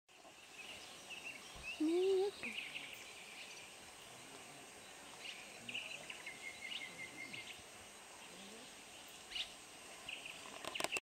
Obur Sincap
Gönen Ilıcasına ikinci kez gittik ve tam dönmek üzere de olsa yine sincap gördük :) Geçen ziyaretimizde üç sincap bir ağaçta oynuyordu. Bu kez tek bir sincap hevesli hevesli bir şey yerken kadraja girdi :))